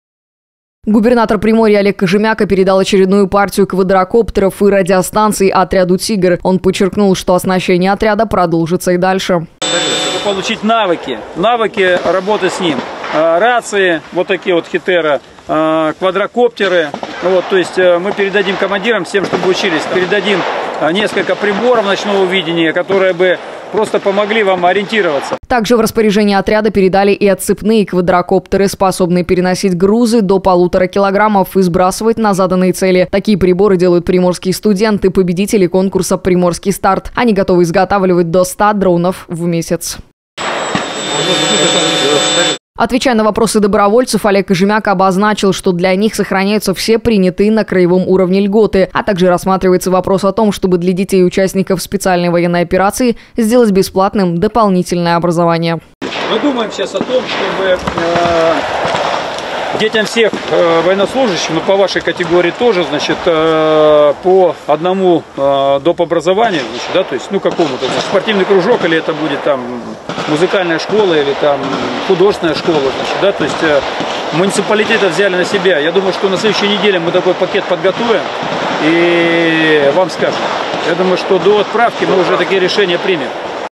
Оснащение добровольцев.
Губернатор Приморья Олег Кожемяко передал очередную партию квадрокоптеров и радиостанций отряду «Тигр».